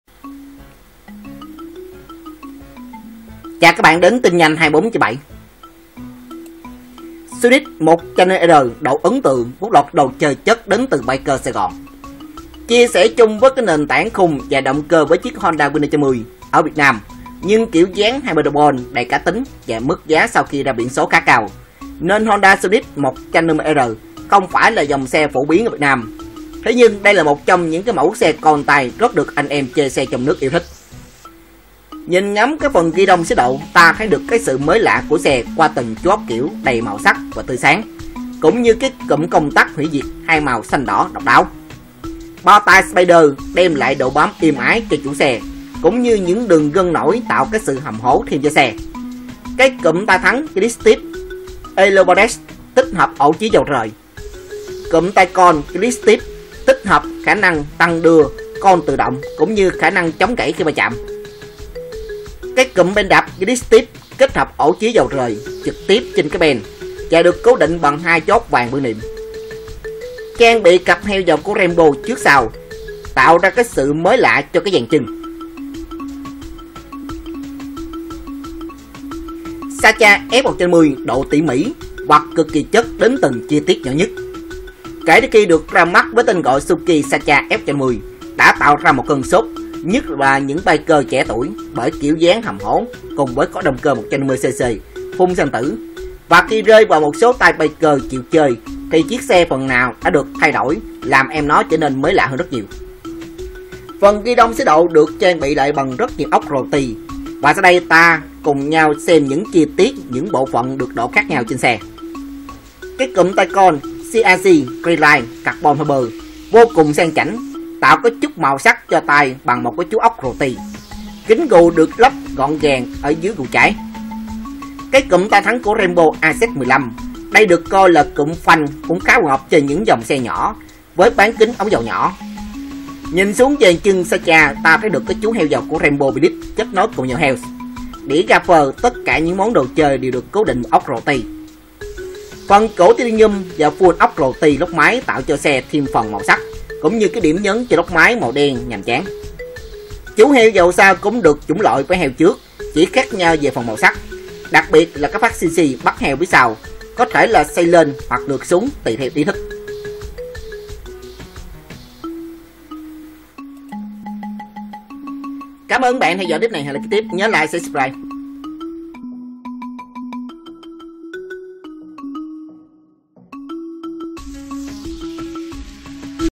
Xe & Phong Cách 24h - Sonic 150R độ ấn tượng với loạt đồ chơi chất của Biker Sài Gòn.
Chia sẻ chung một nền tảng khung và động cơ với Honda Winner 150 tại Việt Nam nhưng với kiểu dáng hyper underbone cá tính và mức giá sau khi "ra biển" khá cao, nên Honda Sonic 150R không phải là dòng xe phổ biến tại Việt Nam, thế nhưng đây cũng là một trong những mẫu xe côn tay rất được anh em chơi xe trong nước yêu thích.